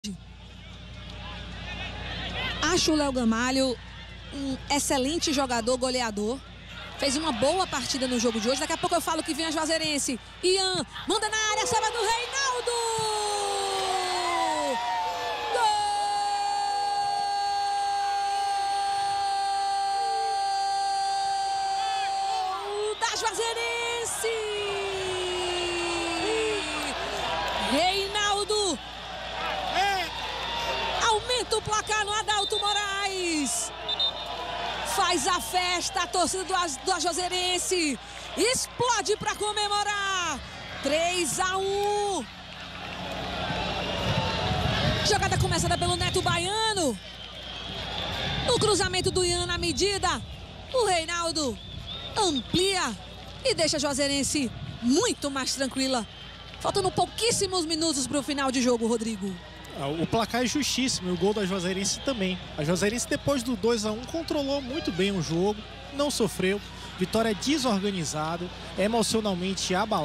VAI CANCÃO! JUAZEIRENSE FAZ 3 A 1 NO VITÓRIA!
Juazeirense vence a primeira no campeoanto.